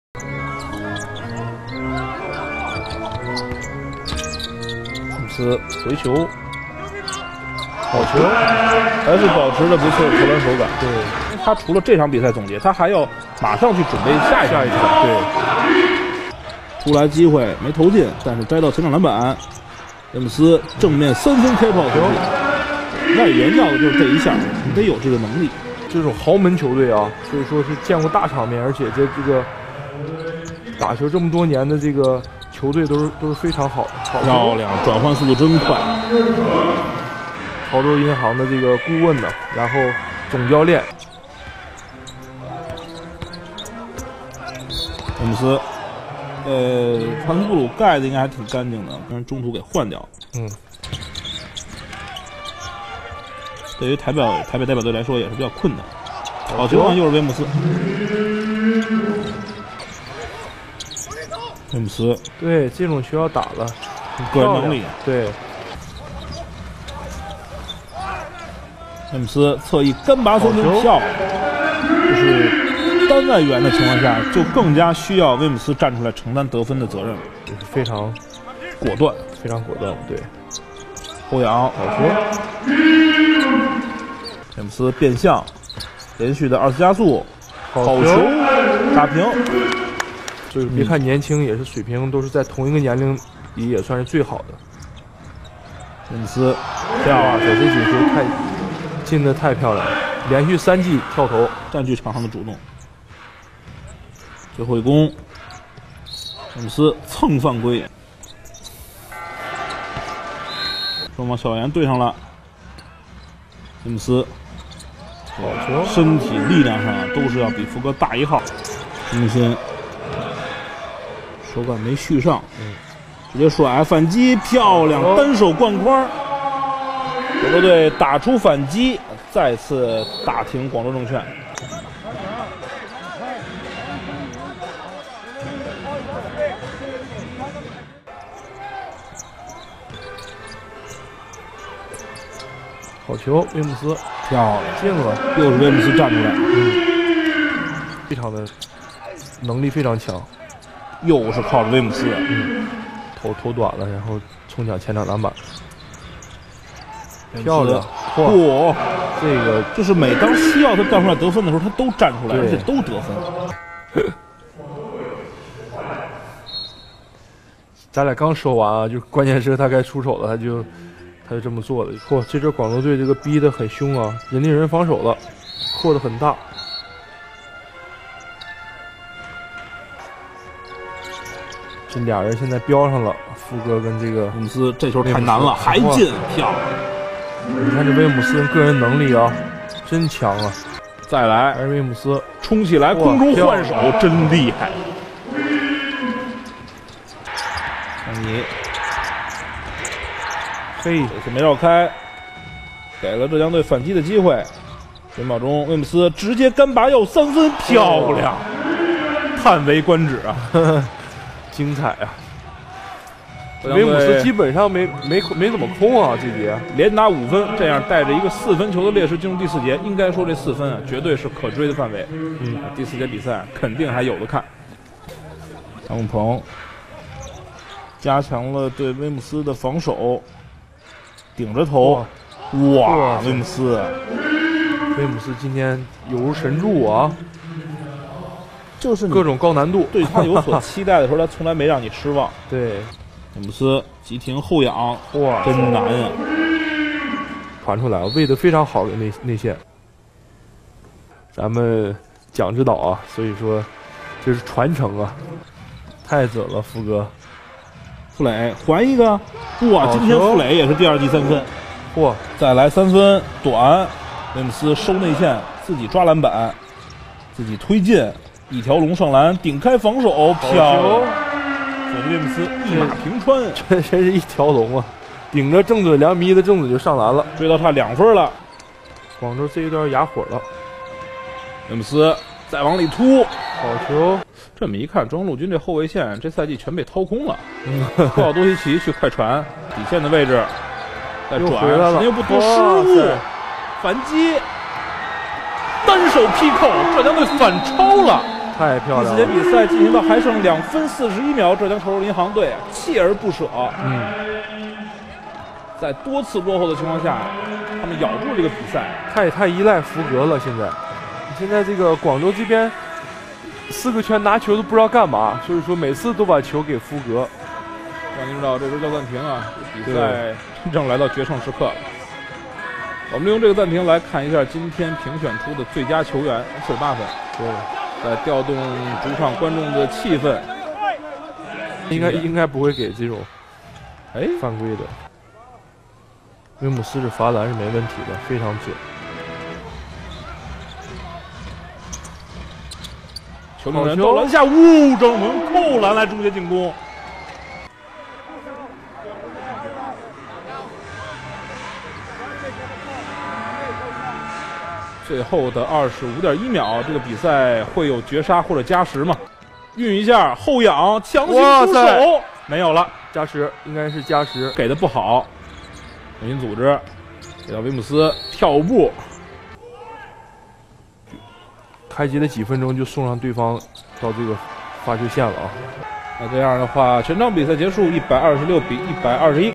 Sonny Weems 54 Pts 10 Rebs 6 Asts Full Highlights vs 广州证券龙狮 (04.02.18) Ferocious Dunk! [1080p]
CBA season about to come to an end but too bad that Zhejiang might be going to the playoffs. But! If they didn't, Toronto Raptors! You know what to do!

btw, I missed a FT bc the camera man missed it.

All clips/photos property of CBA/QQ sports